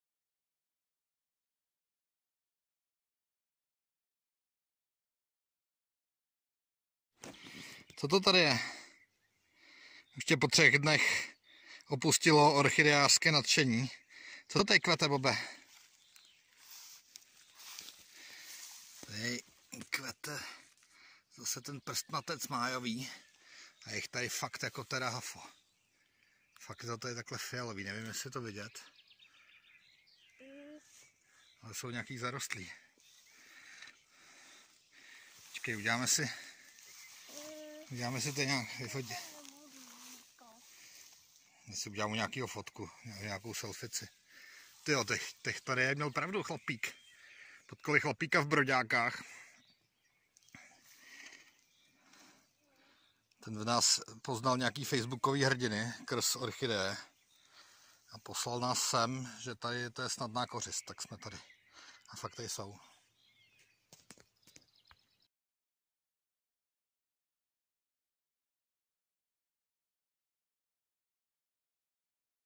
Orchid Hunting, se01ep04: prstnatce májové u silnice v Jílovém na Děčínsku, 6.6.2021
Prstnatce májové "zadarmo" hned u auta u silnice.